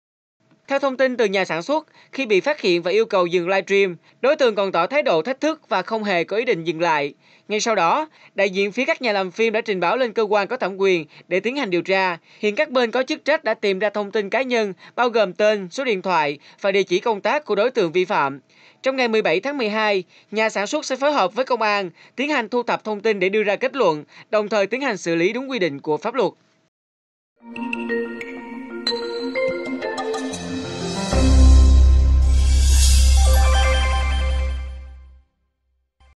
Phẫn nộ khi phim rạp "Gái già lắm chiêu 2" bị livestream bất hợp pháp - SAIGONTV
Phẫn nộ khi phim rạp Gái già lắm chiêu 2 bị livestream bất hợp pháp